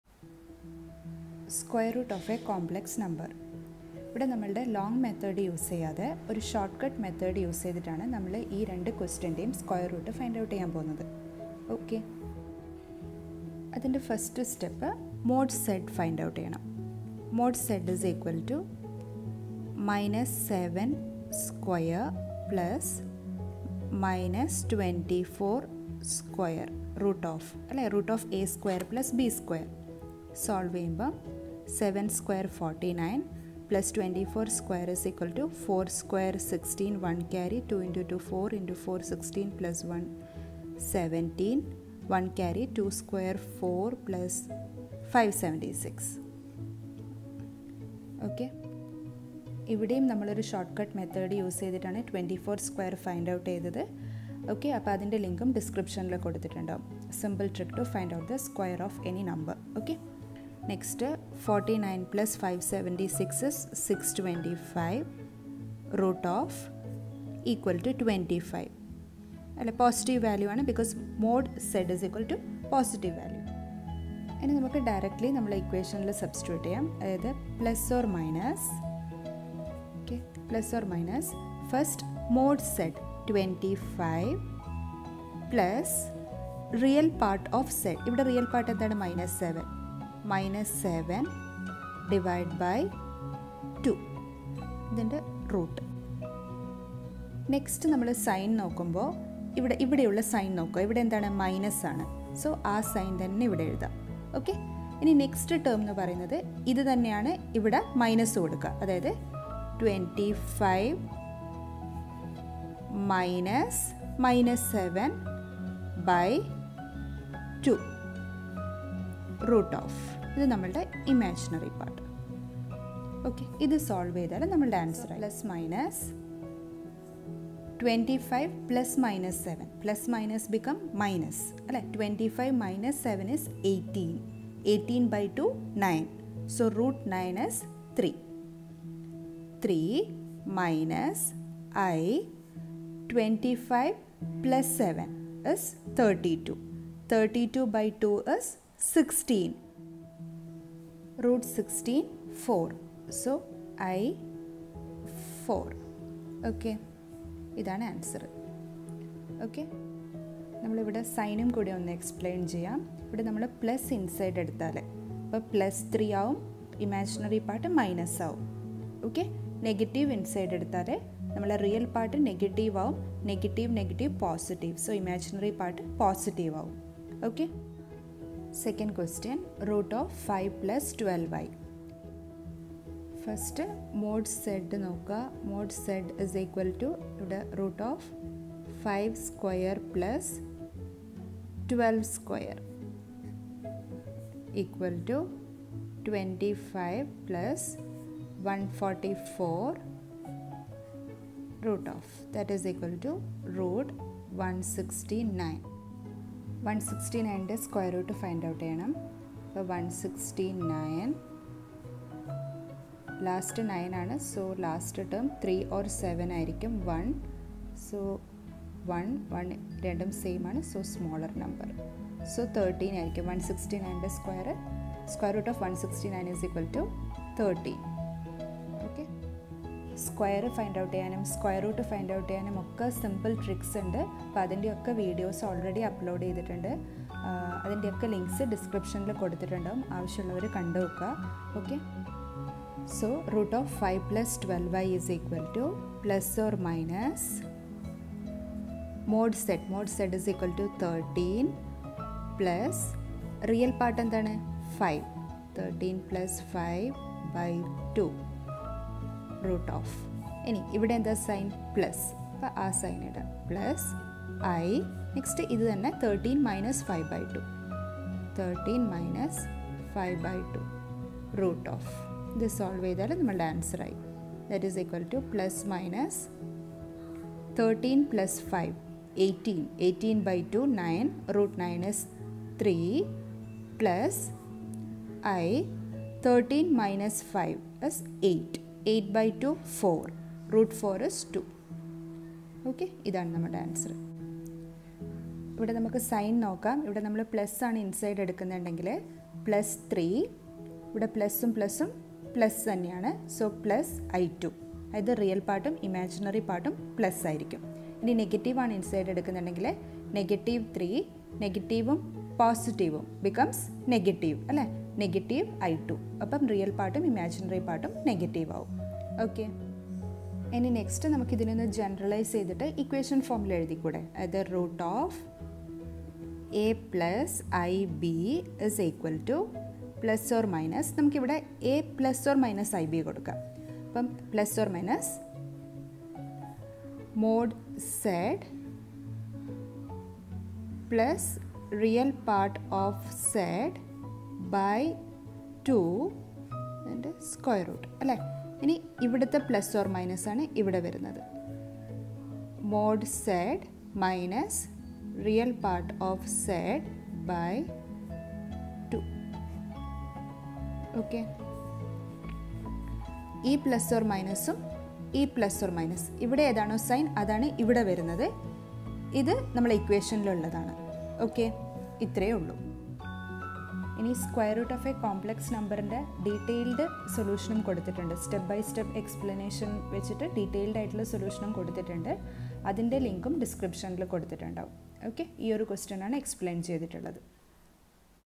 CLASS 11 | SQUARE ROOT OF A COMPLEX NUMBER |SHORT-CUT METHOD | IN MALAYALAM
-square root of a complex number (step by step method )

-SIMPLE TRICK TO FIND OUT THE SQUARE OF ANY 3 DIGIT NUMBER

-SIMPLE TRICK TO FIND THE SQUARE ROOT OF ANY NUMBER